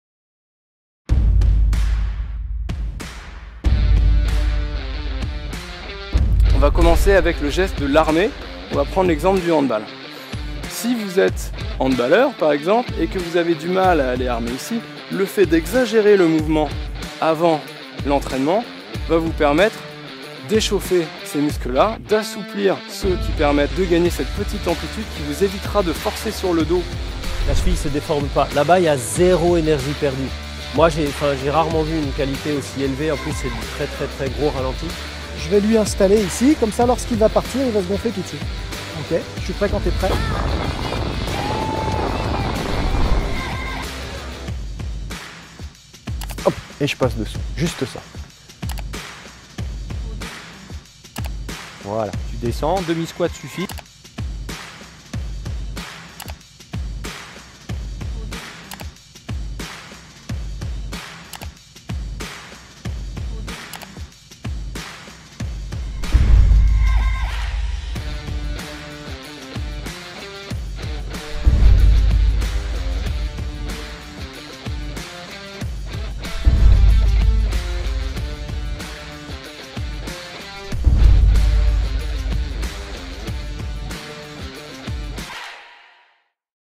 Formation PPRN (Préparation Physique Récupération et Nutrition) by Trans-Faire
Notre organisme de formation Trans-Faire vous propose une formation destinée aux professionnels qui préparent les athlètes dans les sports individuels et collectifs.

Celle-ci est animée par de nombreux spécialistes du sport de haut niveau.

Nouveauté : l'inscription à cette formation vous donne accès au e-campus de TF, plateforme e-Learning des métiers du sport animée par des experts Olympiques et aux infographies de Yann Le Meur sur les dernières nouveautés en science du sport

Pour tout renseignement, nous vous invitons à consulter notre site internet